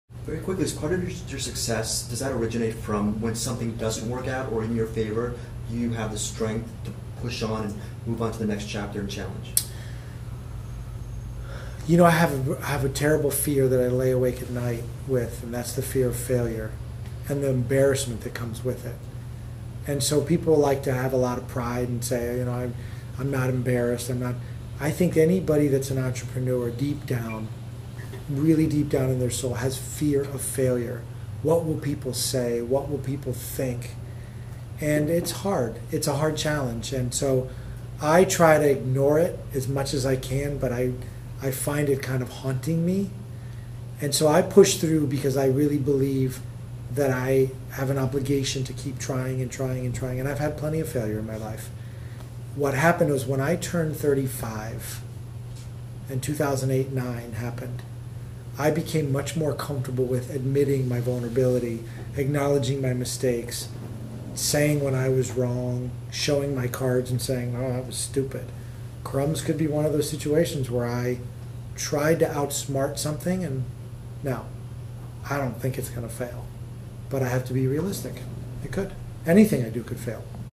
Marcus Lemonis ("The Profit") On Fear of Failure & Moving Forward
Marcus Lemonis, star of CNBC's The Profit, explains how his Fear of Failure motivates him.

To check out my post on Lemonis, please go to my Deepest Dream blog